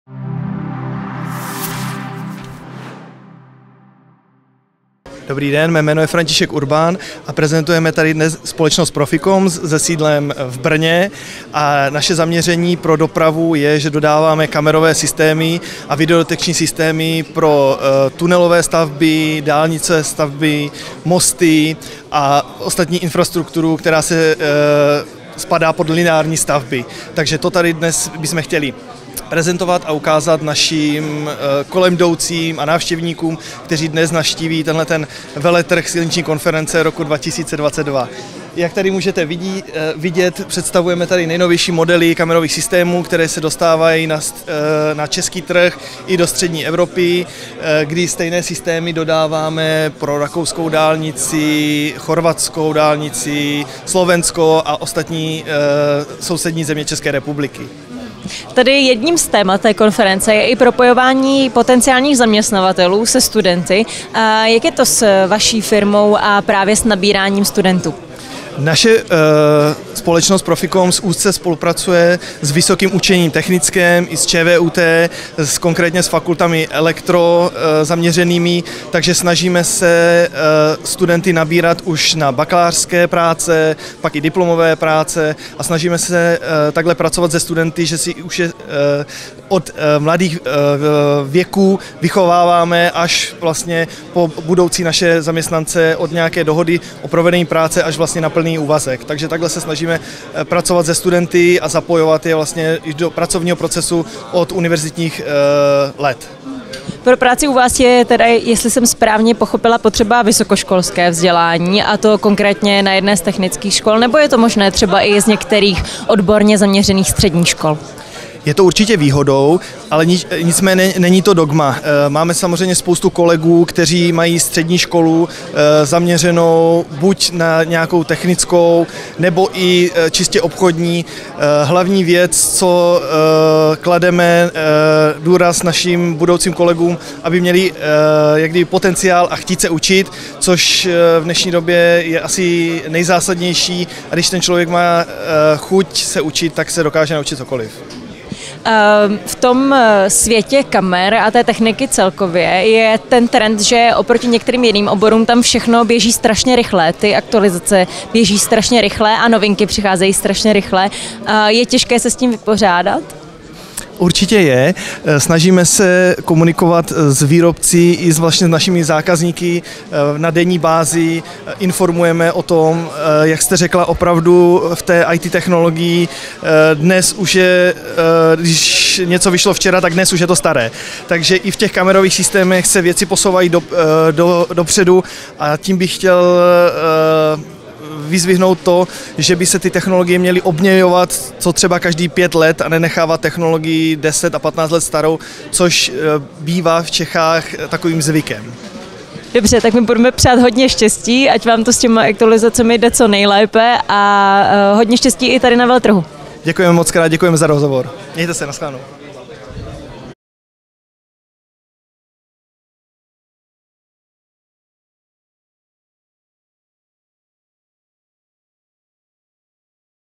Dodáváme kamerové systémy pro silniční stavby a infrastrukturu - František Urban
Spolupracujete s některými vysokými školami, případně s jakými? Do jakých všech zemí dodáváte vaši techniku? František Urban, ředitel společnosti PROFIcomms, byl hostem rozhovoru ze Silniční konference. Moderuje Kateřina Adámková. Připravil Mediální dům Preslova